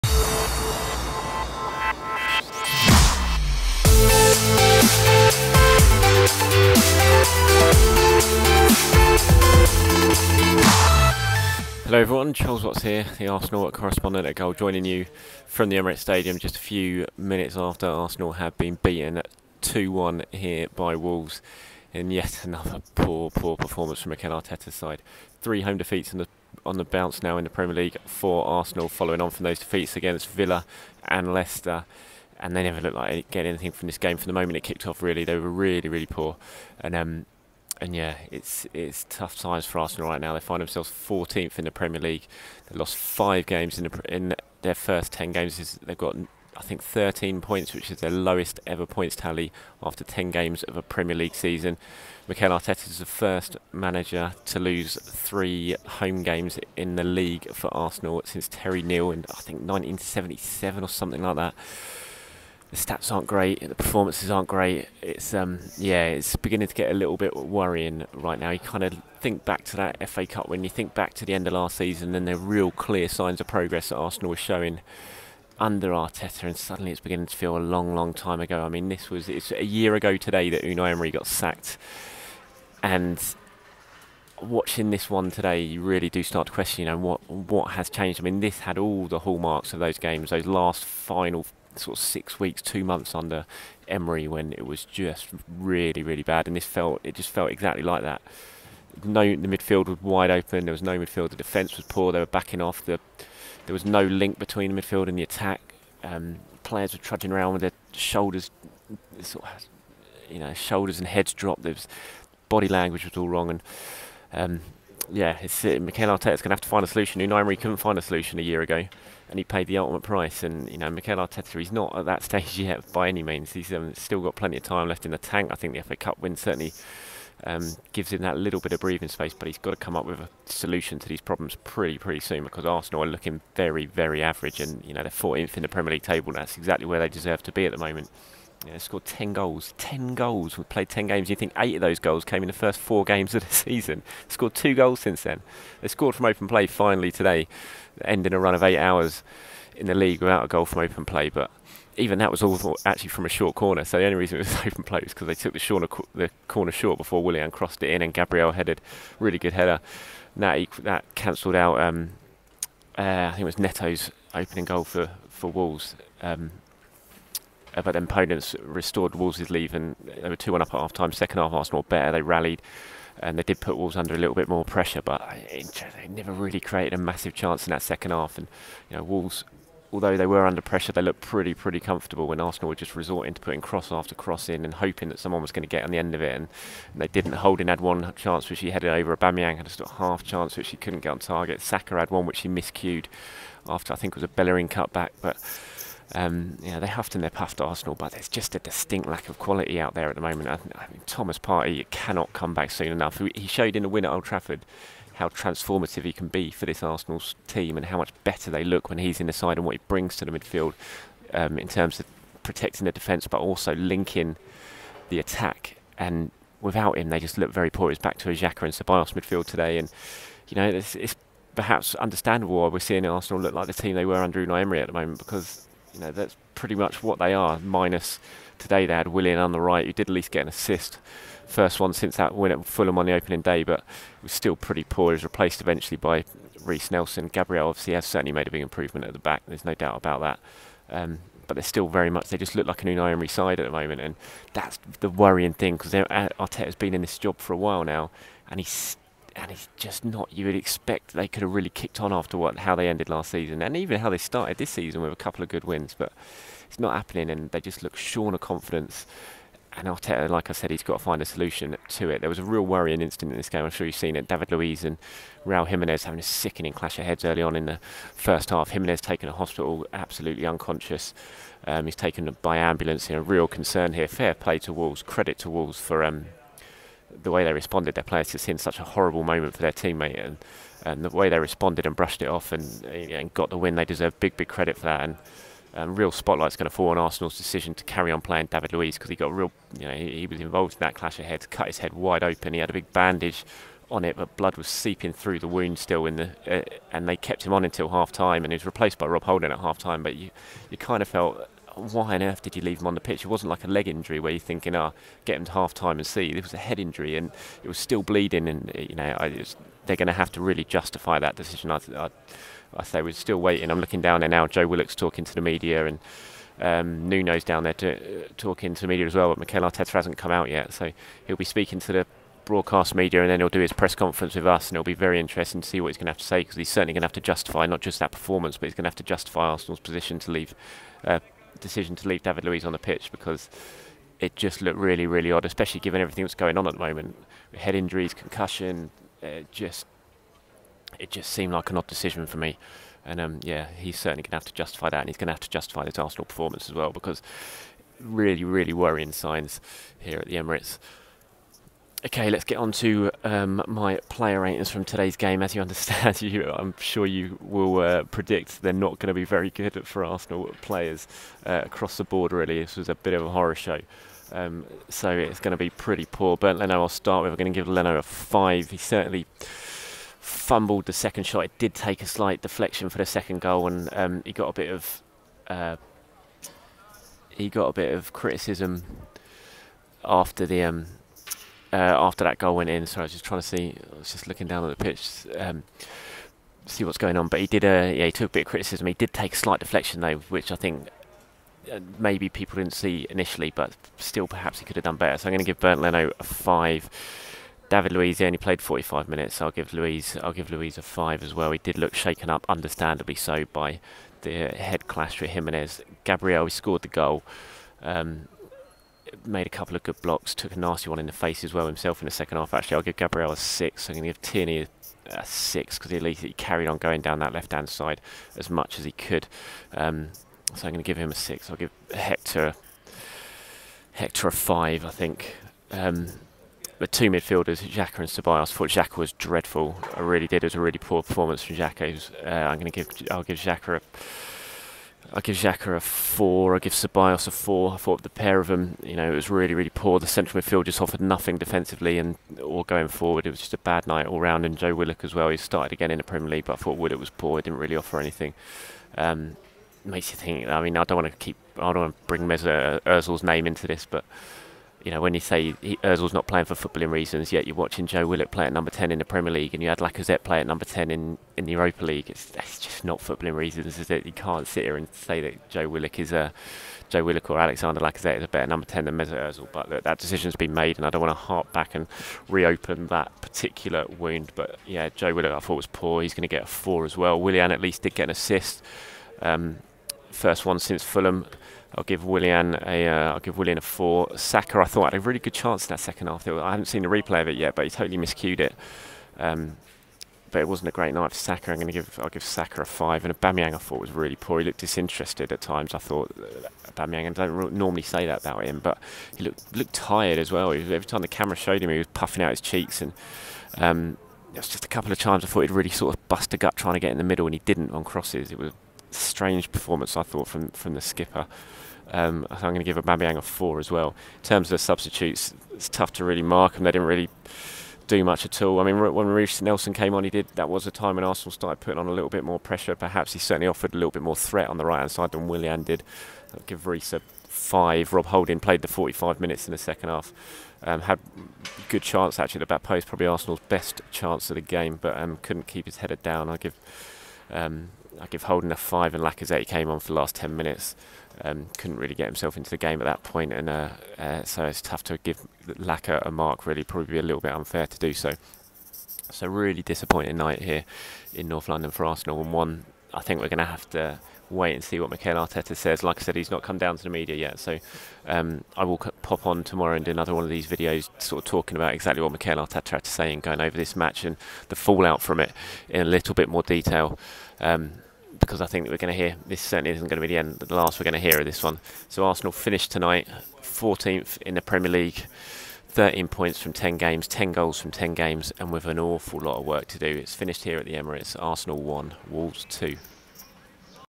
Arsenal 1 Wolves 2: Player ratings and match reaction - Xhaka and Ceballos awful, Aubameyang poor
I react to Arsenal's 2-1 defeat by Wolves in the Premier League and give my player ratings.